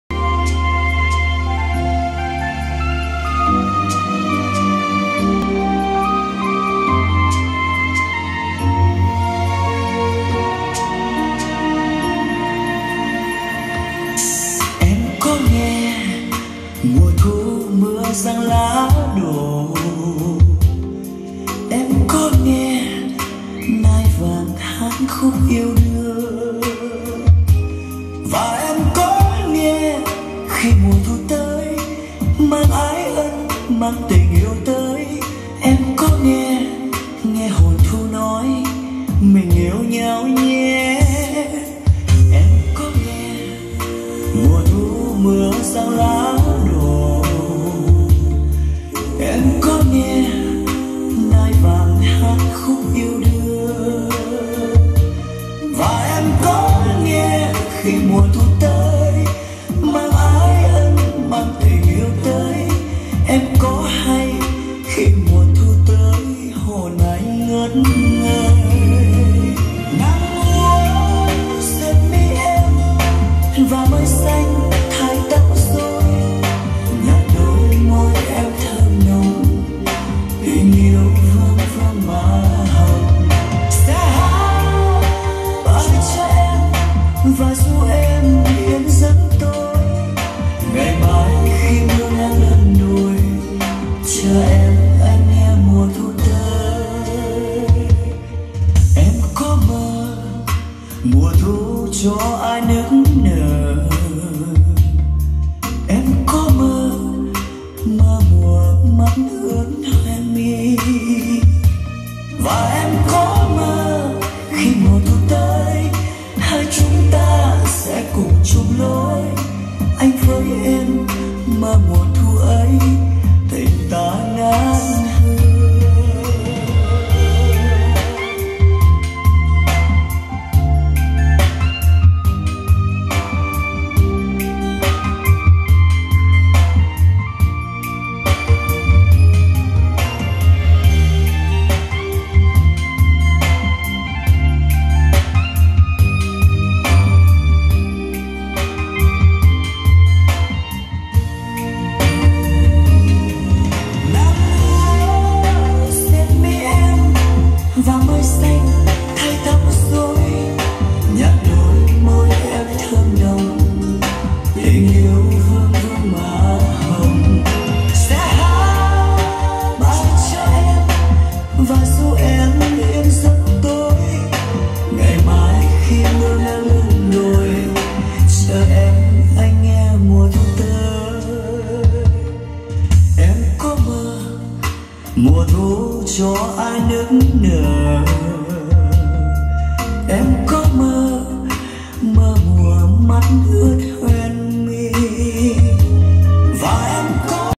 LOA DENON SC 106 GIÁ: ĐÃ BÁN . ĐT: 0904088272
Thùng gỗ lạng nặng trịch góc cạnh đẹp nguyên zin
  Loa bãi tuyển loa đều đẹp suất sắc
 Loa bass 30 gân cao su mềm nhũn,  trung giấy,  2 trep giấy
 Đang ghép với amli sansui au 7500
   Tiếng bass uy lực mạnh mẽ rõ lời sáng chi tiết

   Ai quan tâm alo, zalo biết thêm thông tin